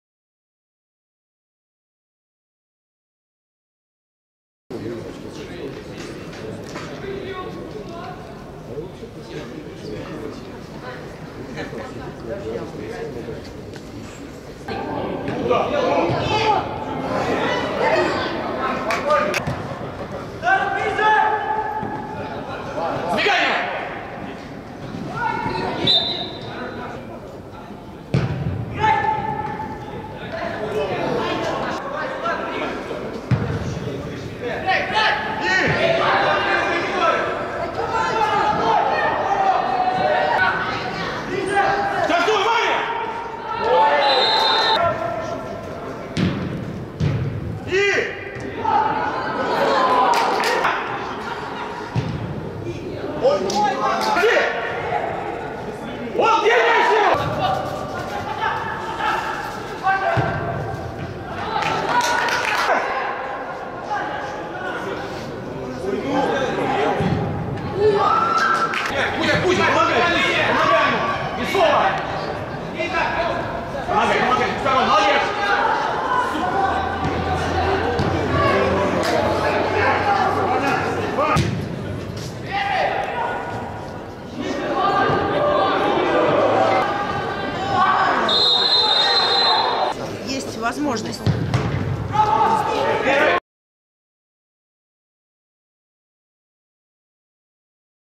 Обзор матча "Спартак" - «Динамо» (команды 2008 г. р.) 0:0
Зимнее первенство Москвы | 7-й тур
Команды 2008 года рождения
«Спартак» — «Динамо» — 0:0